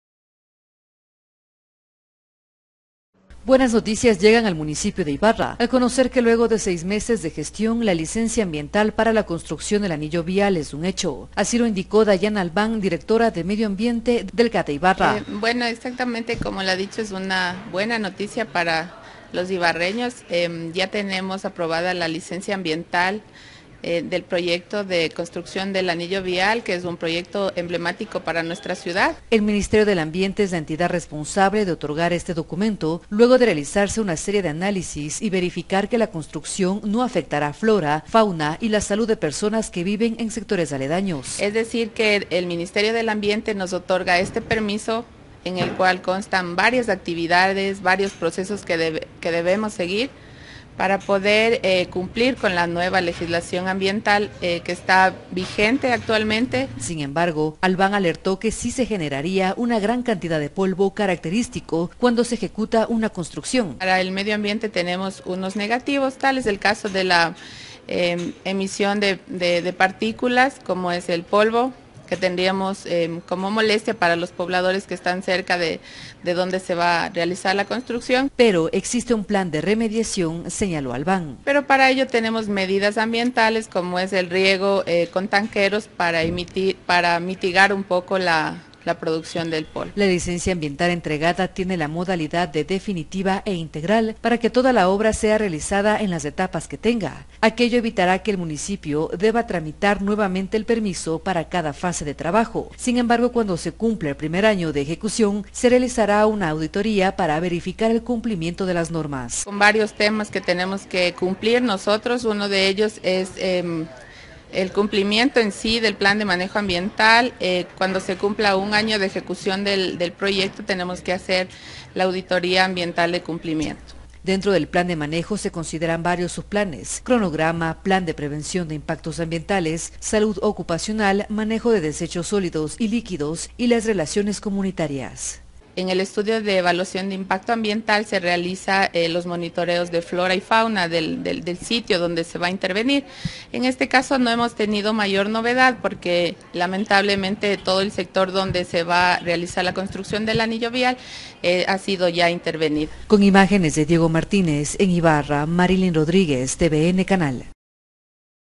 Licencia para la construcción del anillo vial en Ibarra
El Municipio de Ibarra recibió la licencia ambiental para la construcción del anillo vial, este documento permitirá completar con mayor facilidad los requisitos que exige el Banco Mundial para conceder un crédito.